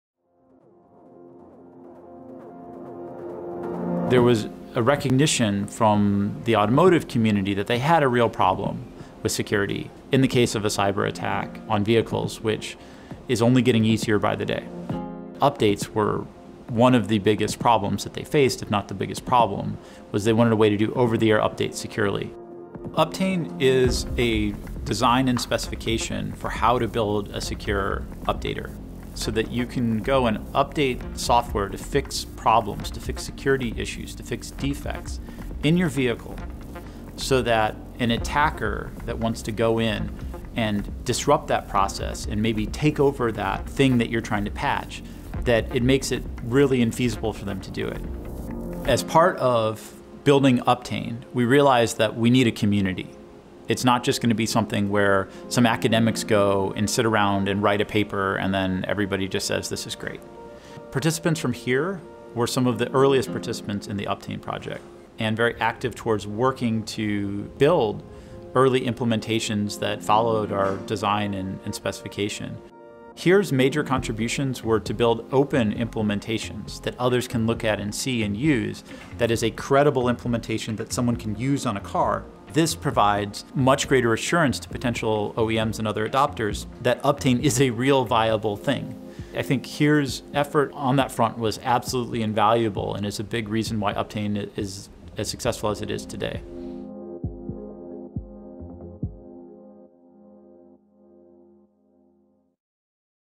Uptane - A Chief Researcher’s journey into connected vehicle security
NYU Tandon School of Engineering professor Justin Cappos is a computer scientist and cybersecurity expert. See how he harnessed the power of open-source development and security expertise from government, industry and academia to solve real-world security problems in practice.